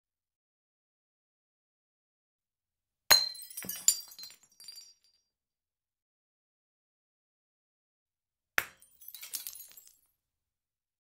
Dropping and Breaking Coffee Cup on Wooden Floor
Dropping and Breaking Coffee Cup on Wooden Floor · Digiffects Sound Effects Library

Dishes and Plates Sounds